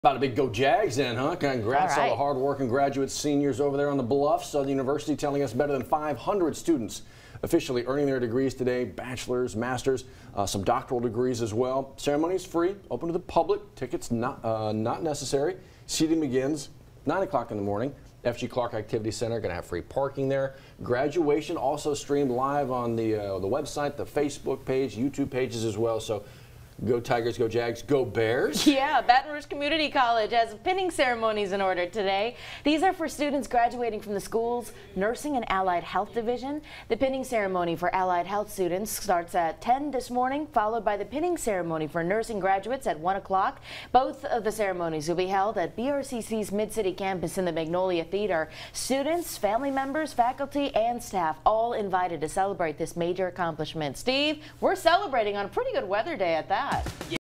Geaux Jags! 600 students will graduate during Southern's fall commencement
Graduation will be held at 10 a.m. in the F.G. Clark Activity Center.